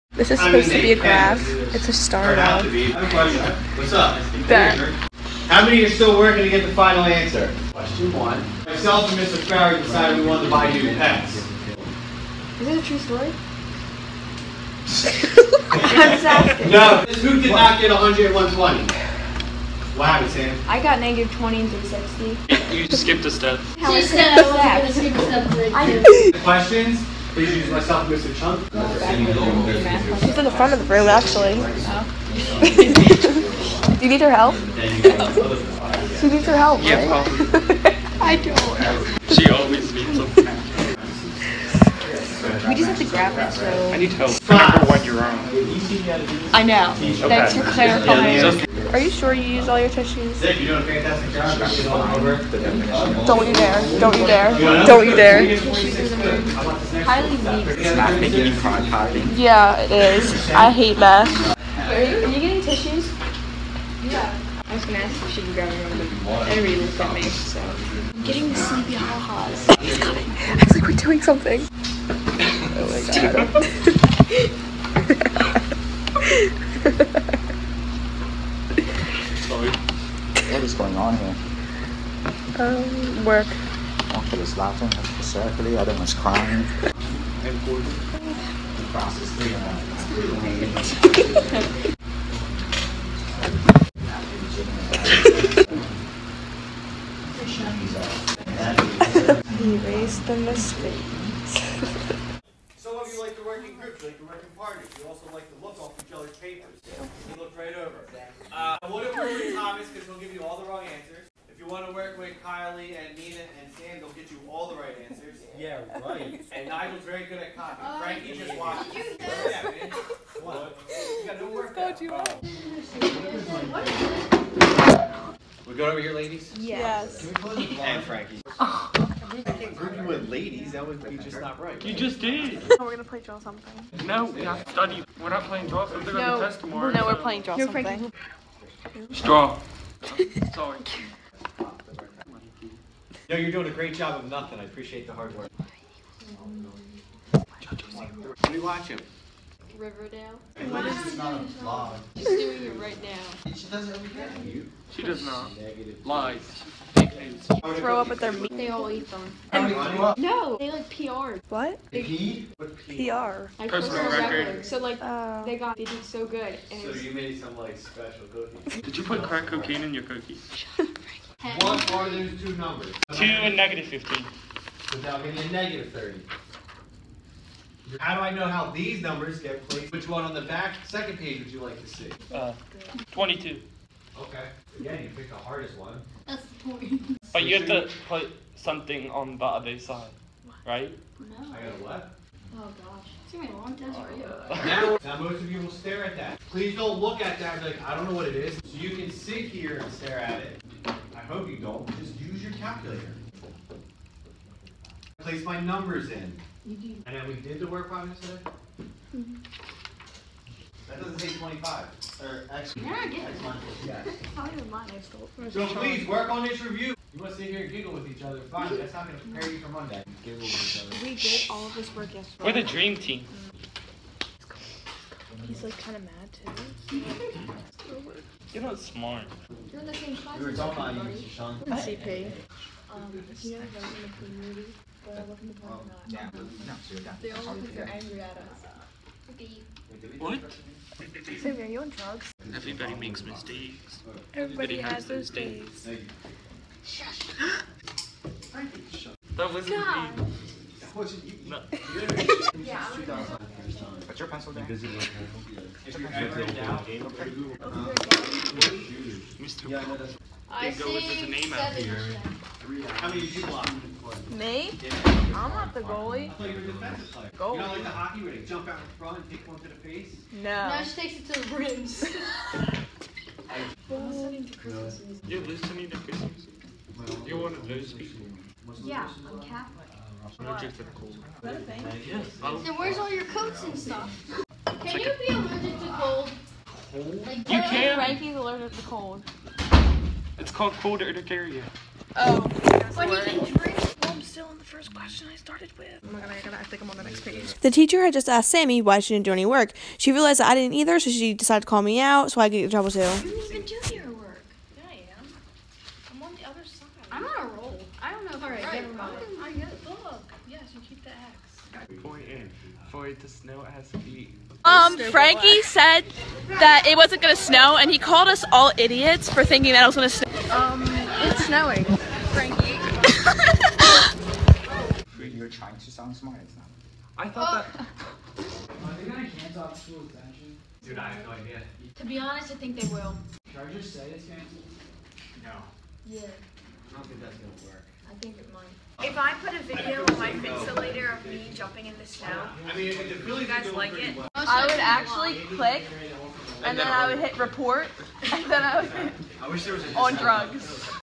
I vlogged my math class for 3 days
Hello these are me and my friends in math class... we have no idea what’s going on!